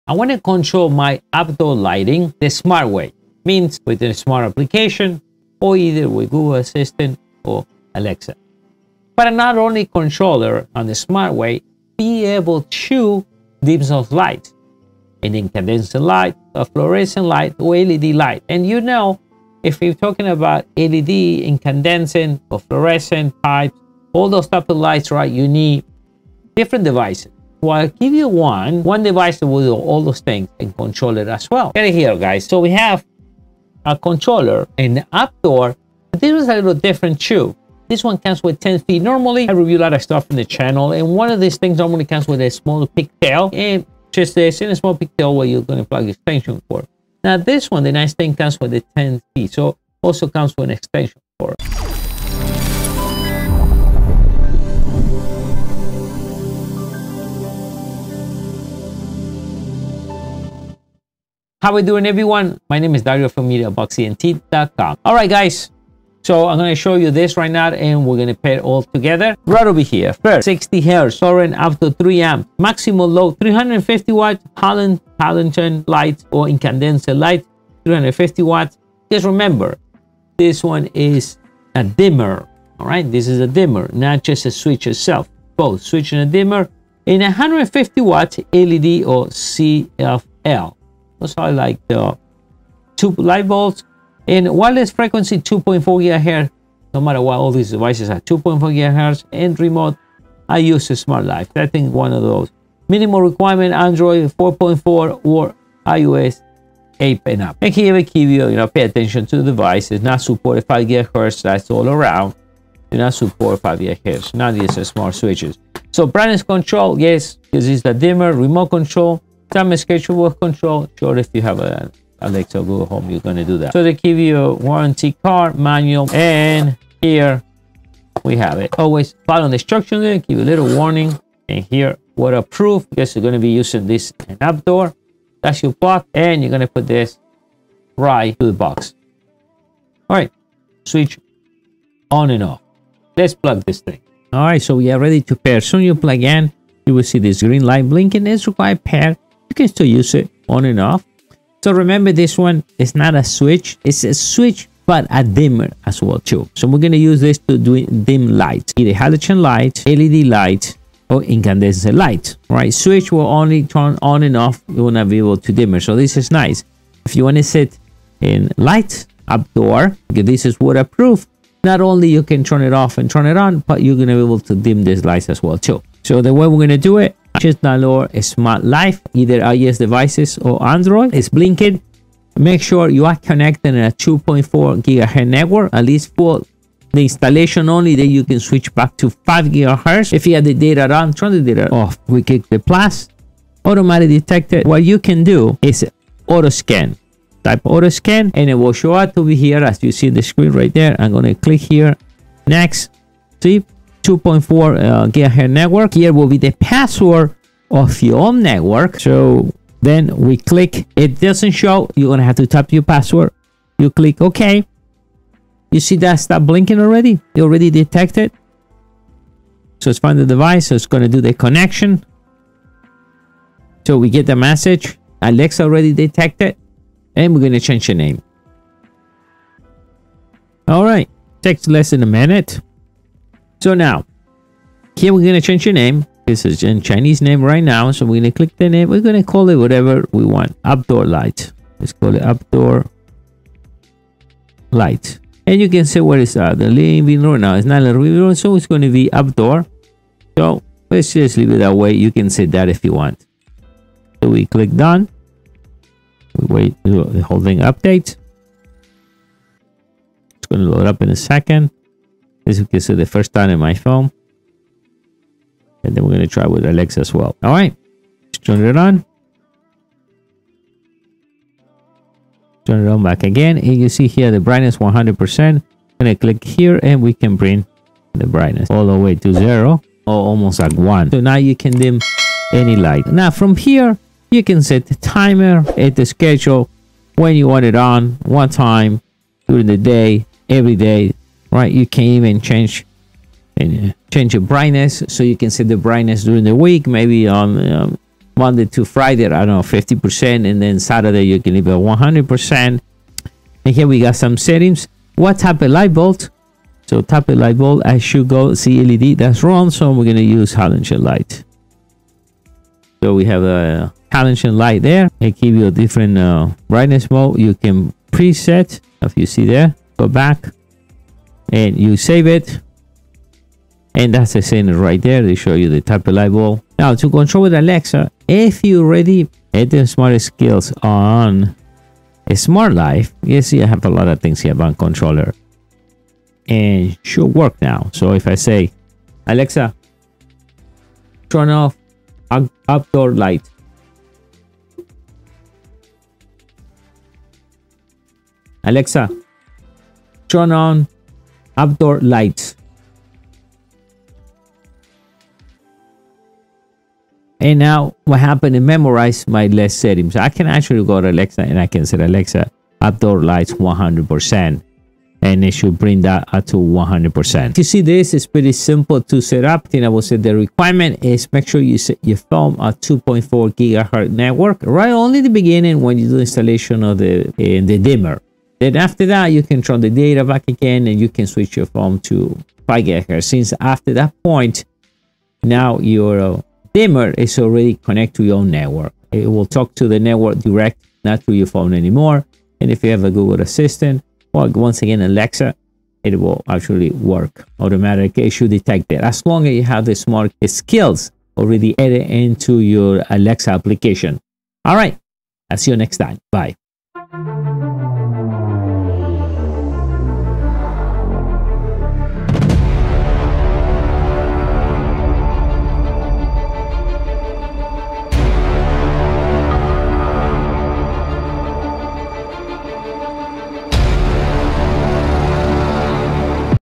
Review: WTESKE Outdoor Dimmer
Outdoor Dimmer, Wteske Smart Light Dimmer with 10ft Extension Cord, Compatible with Alexa Google Home, WiFi Out, Remote Control, 350W Dimming, IP44 Waterproof for Dimmable String Lights.

Outdoor Dimmer